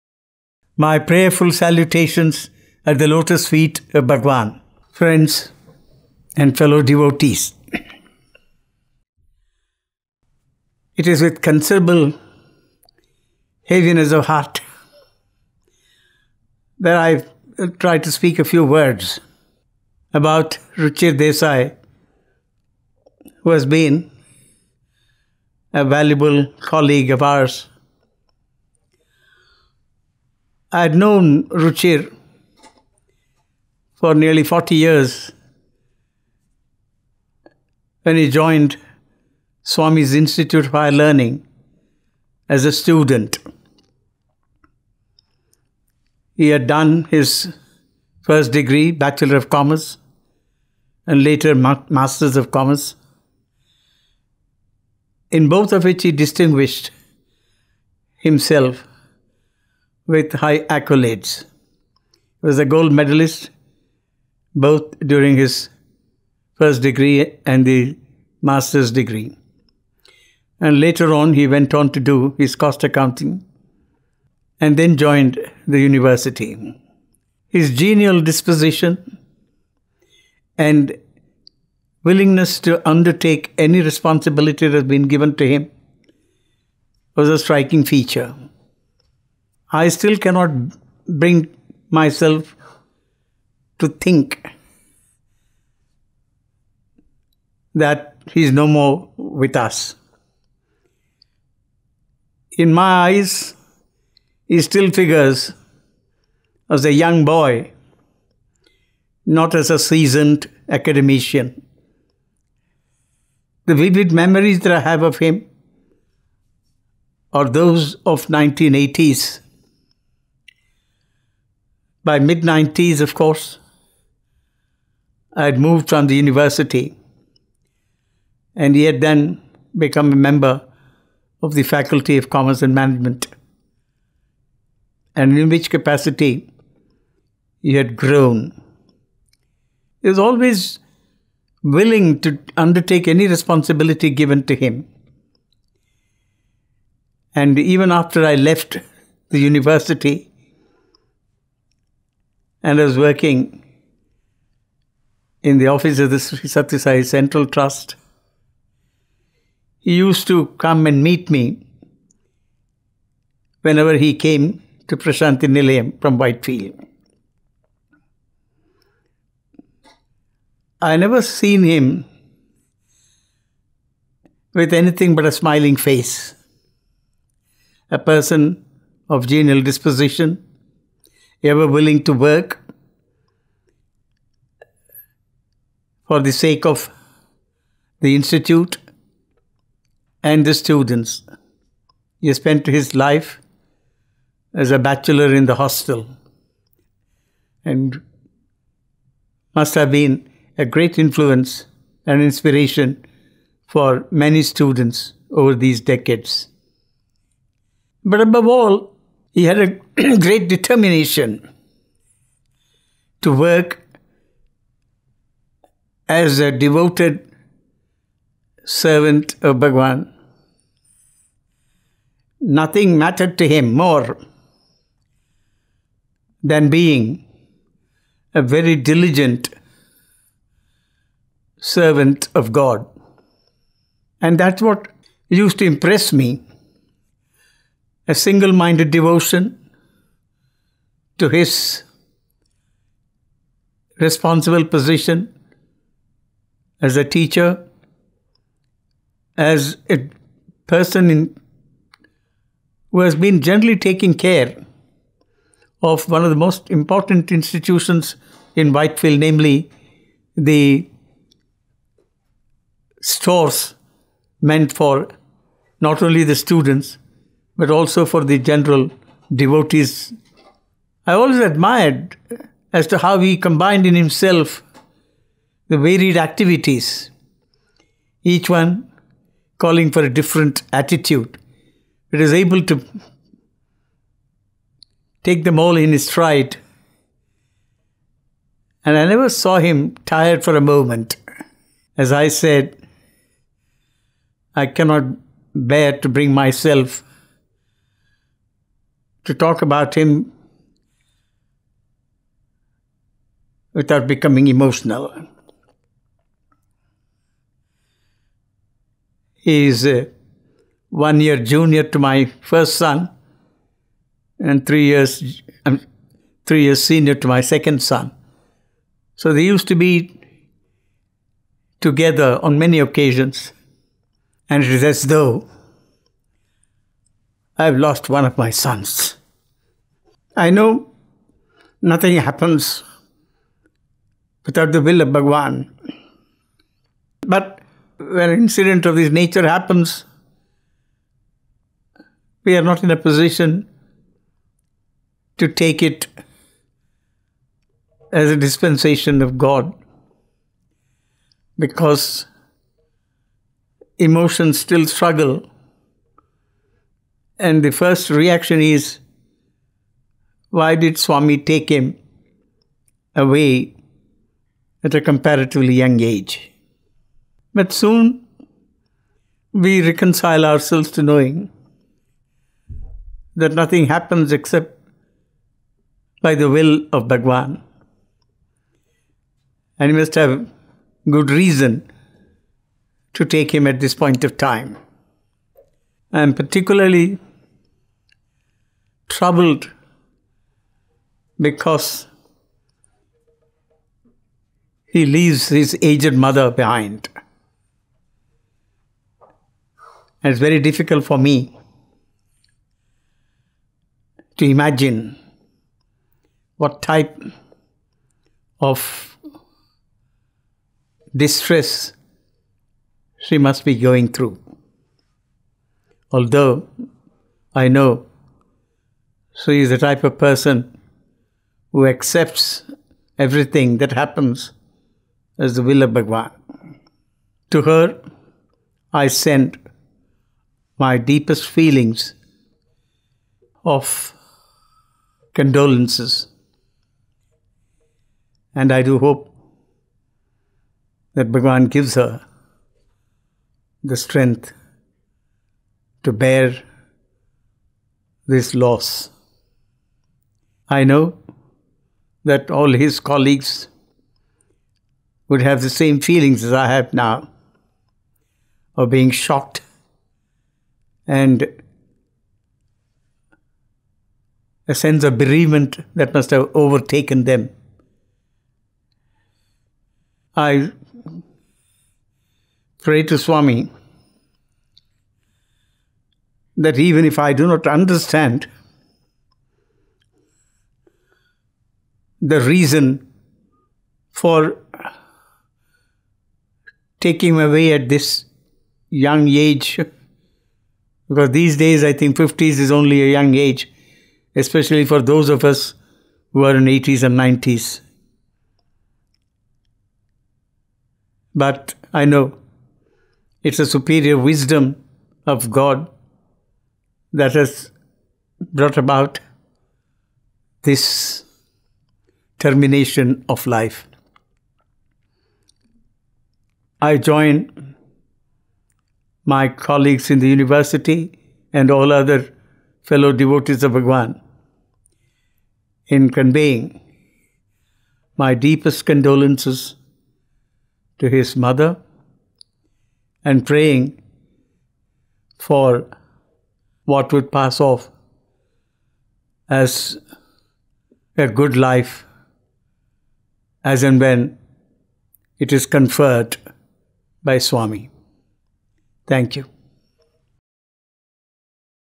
In Loving Memory of Sri Ruchir Desai – A Tribute by Sri K. Chakravarthi, Chancellor, SSSIHL
The Sri Sathya Sai Institute of Higher Learning expresses its’ heartfelt condolences on the unexpected demise of a valued member Sri Ruchir Desai, Associate Professor, Department of Management and Commerce, Brindavan campus. Ruchir, merged in the Lotus Feet of our Beloved Bhagawan Baba at 5.55 pm on  Monday,17 May 2021 after a brief illness.

A distinguished alumnus of the Institute (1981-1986) and a teacher par excellence, Sri Ruchir Desai was appointed by Bhagawan as a teacher in the Brindavan Campus in 1986. A serious academician with a bias for academic rigour, Ruchir was one of the finest accounting faculty the Institute has seen. Over the decades, he has been a great inspiration not just to hundreds of his students, but to one and all who have known him.

In short video messages, the Chancellor, the Vice-Chancellor, the Registrar, and the Controller of Examinations expressed their deepest condolences, heartfelt prayers, and thoughts on this bright son of Mother Sai: